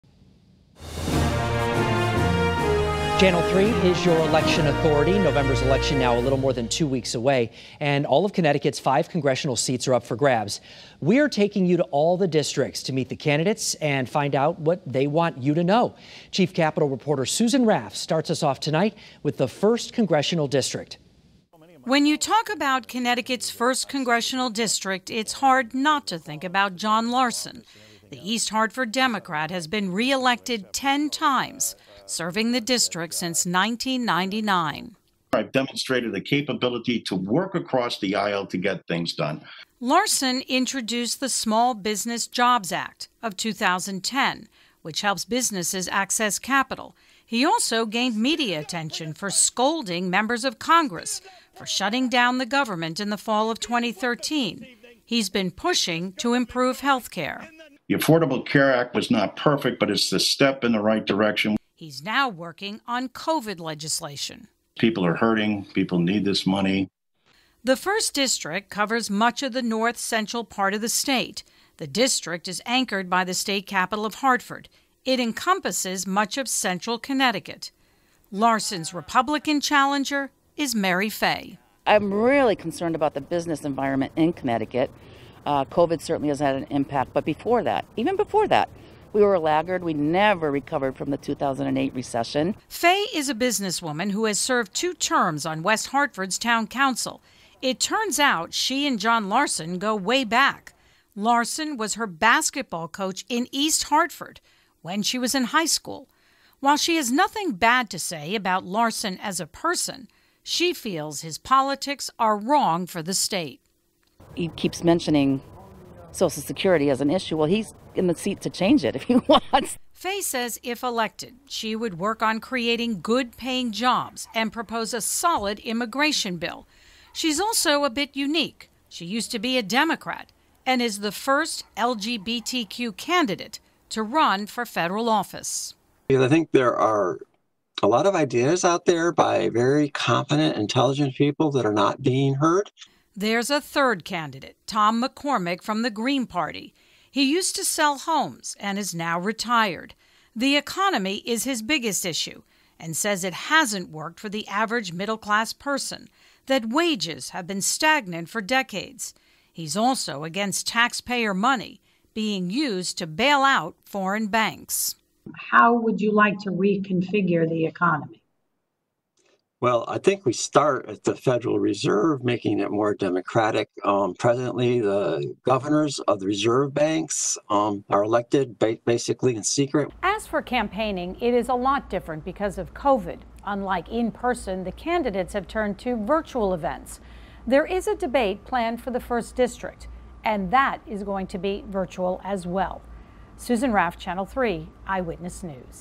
VIDEO: Meet the candidates of the First Congressional District
November’s election is a little more than two weeks away and all of Connecticut’s five Congressional seats are up for grabs.